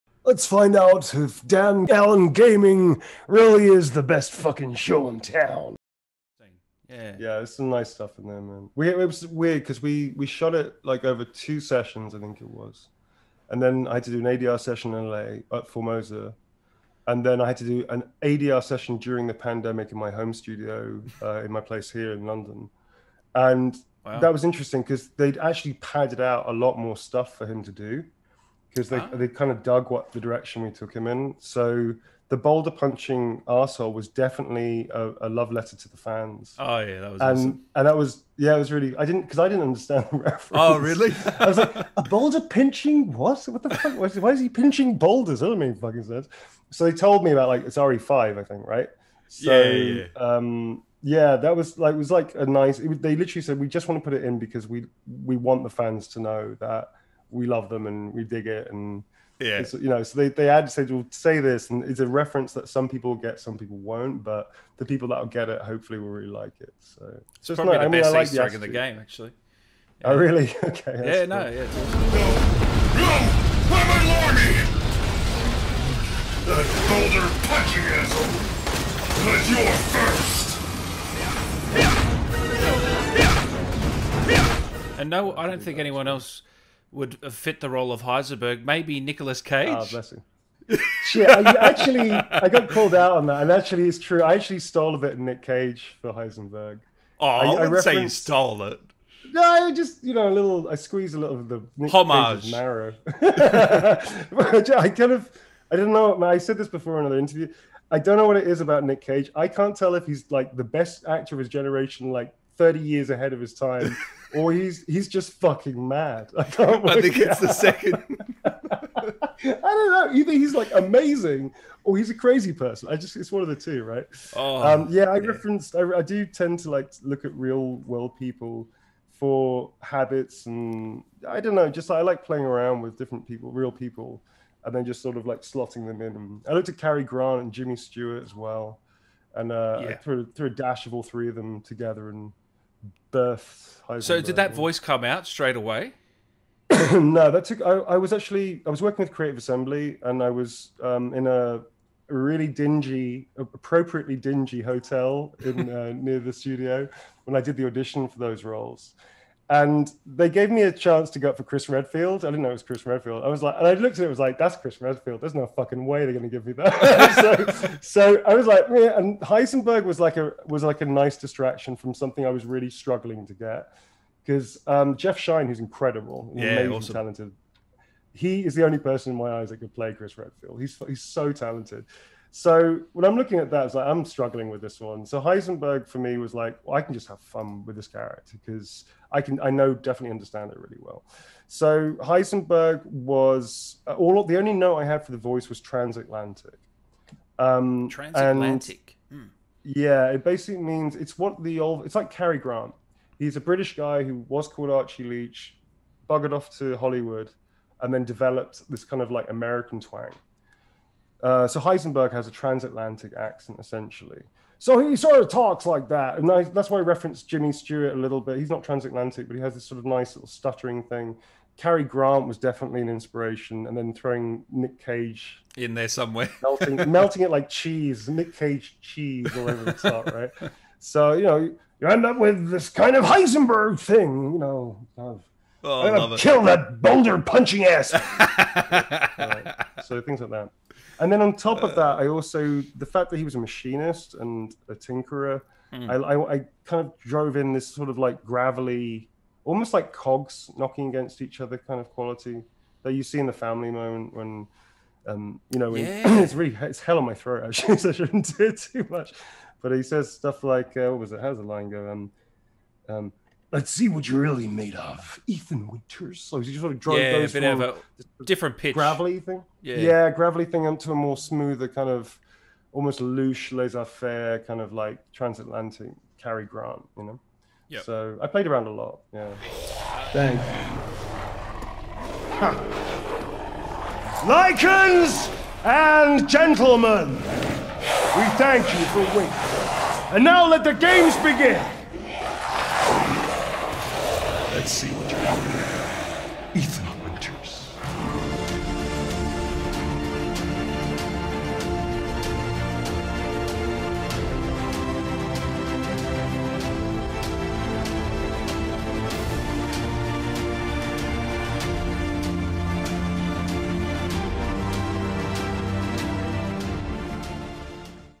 RESIDENT EVIL 8 VILLAGE - Heisenberg Actor on the Chris Boulder Punching Easter Egg
Chris Roasted For Punching Boulder Scene. Here is a snippet from my conversation with the incredible, Neil Newbon, the voice and performance capture for Kalr Heisenberg in RE8 Village.

RESIDENT EVIL 8 VILLAGE - Heisenberg Actor talks Chris Boulder Punching Easter Egg